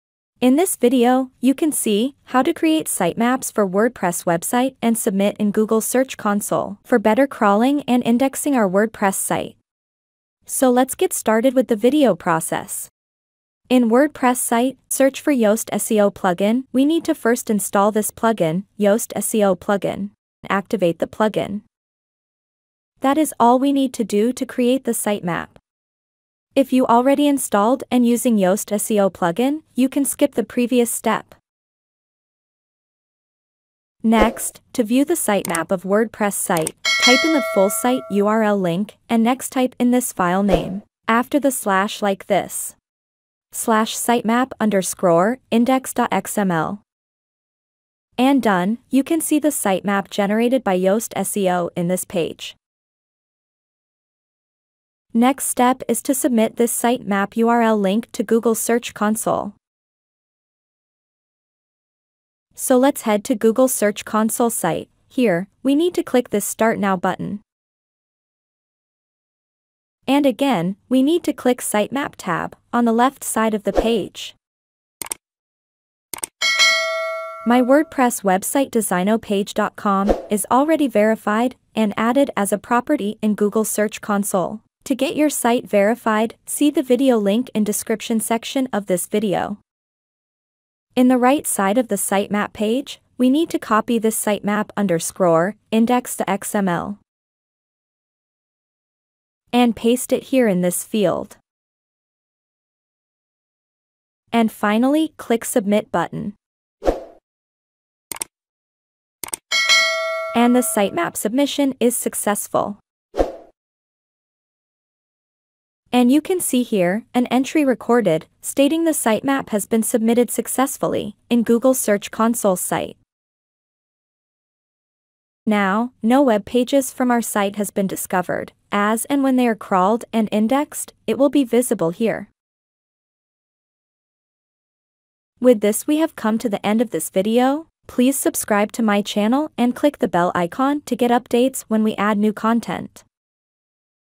Create Sitemap For Wordpress Website And Submit In Google Search Console in 2023
Create Sitemap for wordpress site and submit in google search console to crawl and index the wordpress pages and posts for better performance and appearing on search engine or SERP